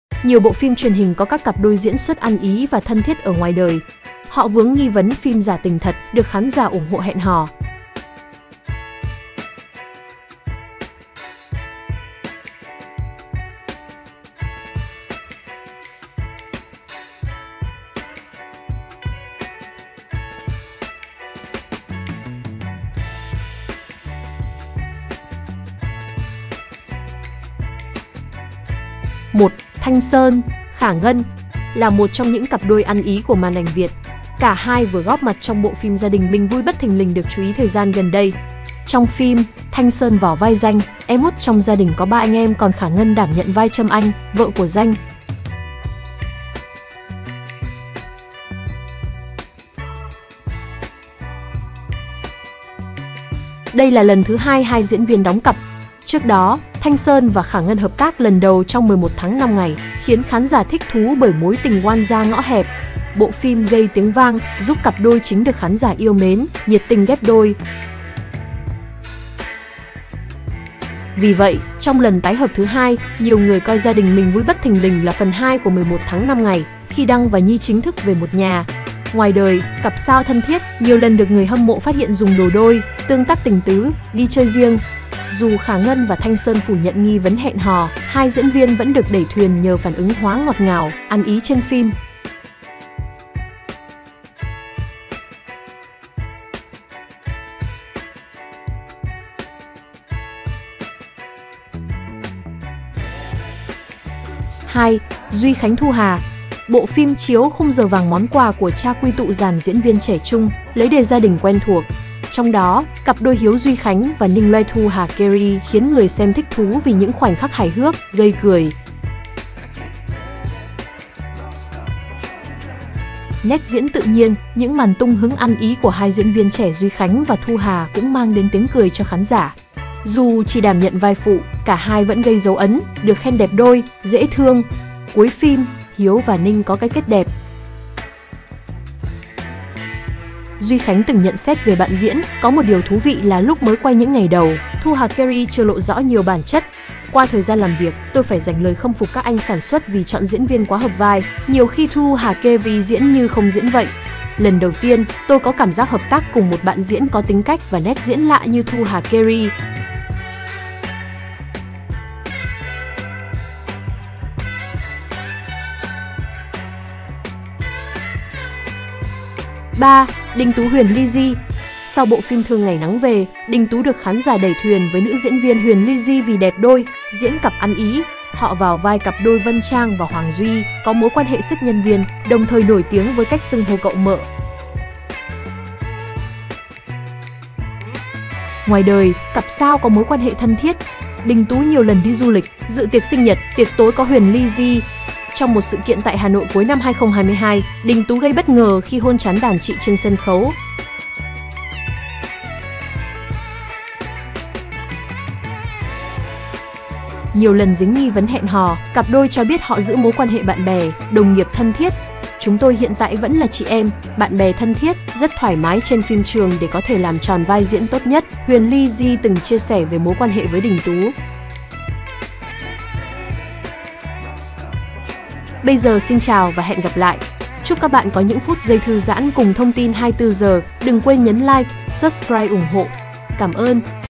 Khả Ngân - Thanh Sơn và những cặp đôi được đẩy thuyền trên sóng giờ vàng.
Nhiều bộ phim truyền hình có các cặp đôi diễn xuất ăn ý và thân thiết ở ngoài đời. Họ vướng nghi vấn “phim giả tình thật”, được khán giả ủng hộ hẹn hò.